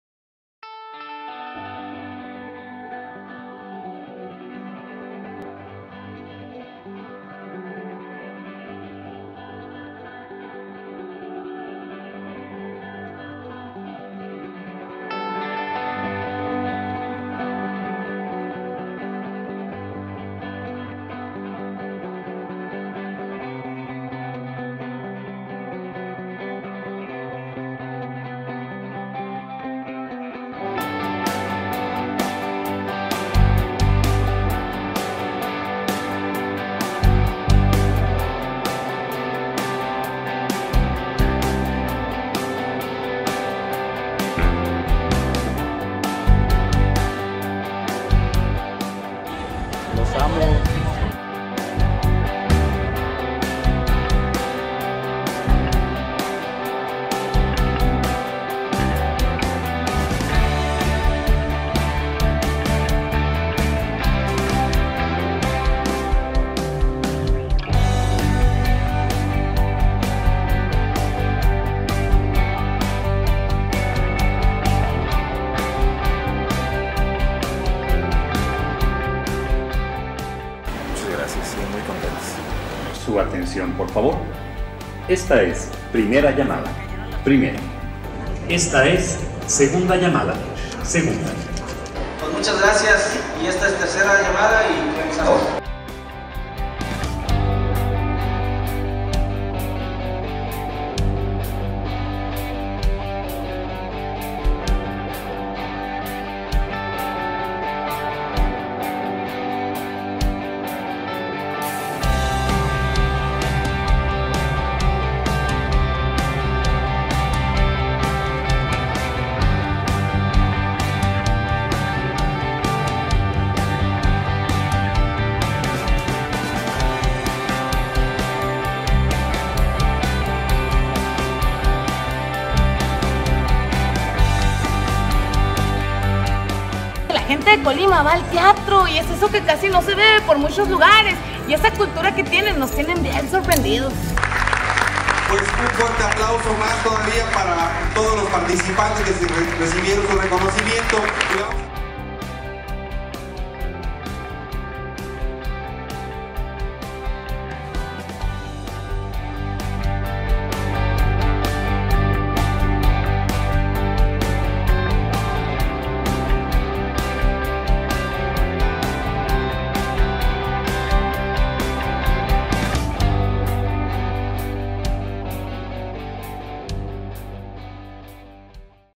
Resumen del Mes Colimense del Teatro 2016
Todo lo que se vivió durante junio, Mes Colimense del Teatro 2016.